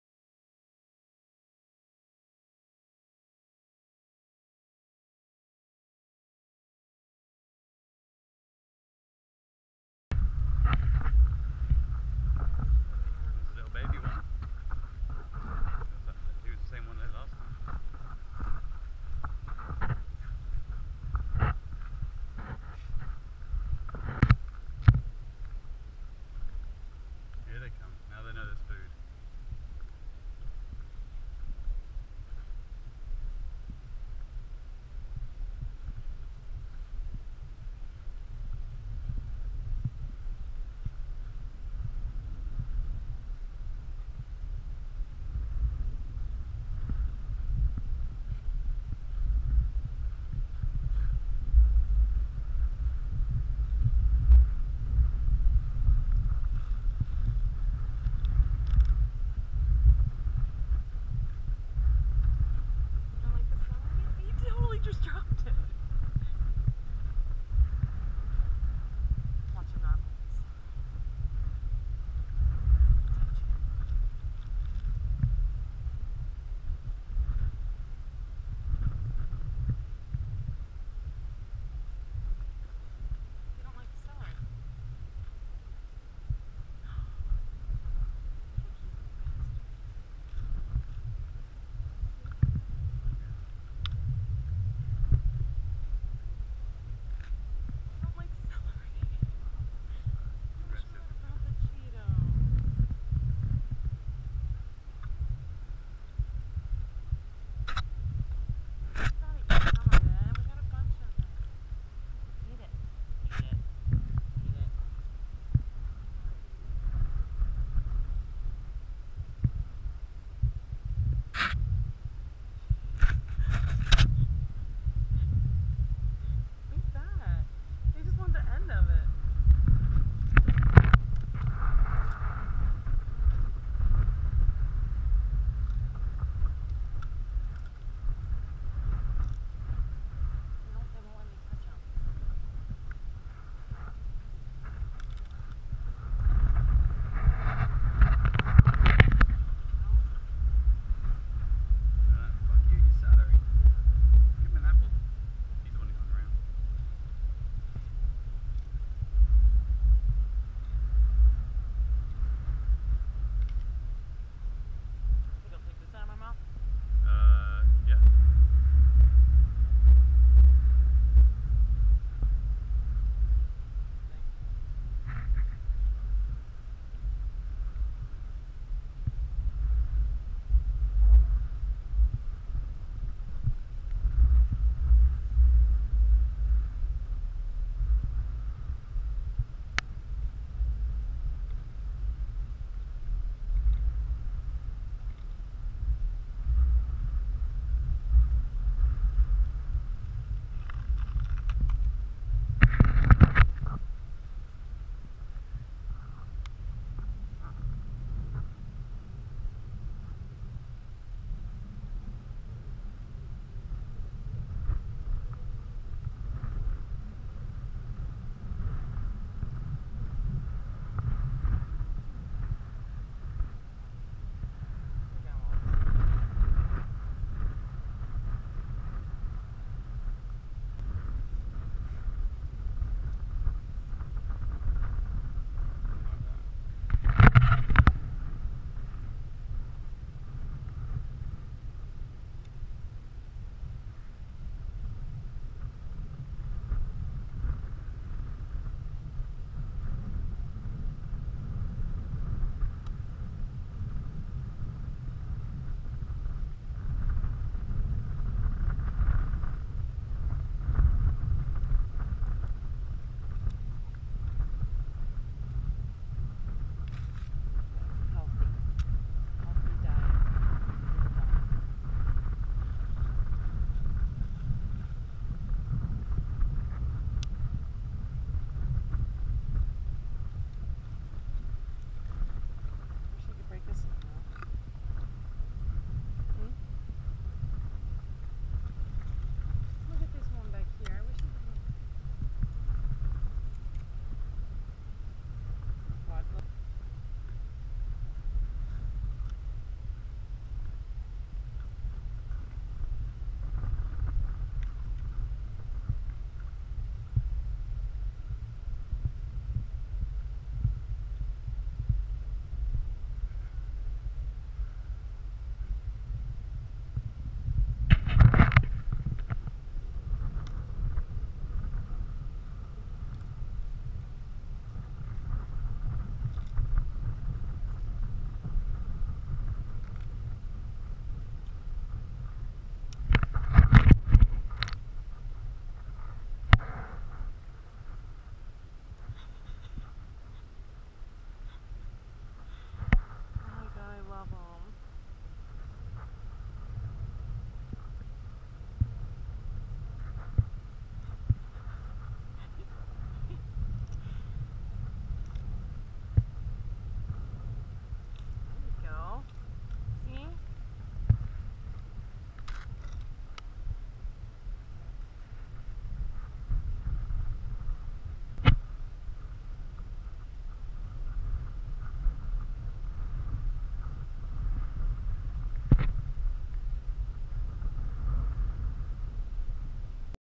Feeding wild Donkeys in Aruba.
We have been looking for these guys for a couple of weeks.